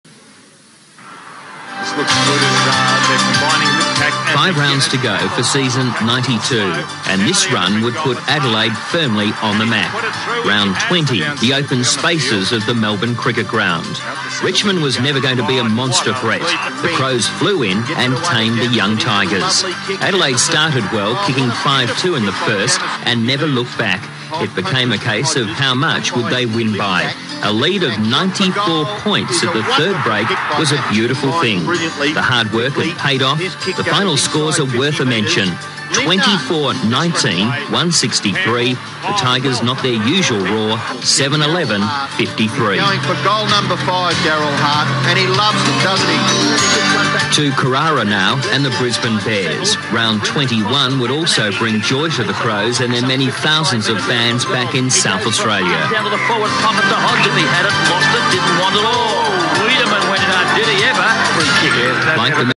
Richmond VS Adelaide Crows 1992 Darel Hart and Rod Jameson outstanding
1992 AFL Highlights form the Richmond VS Adelaide Crows match. Darel Hart and Rod Jameson outstanding.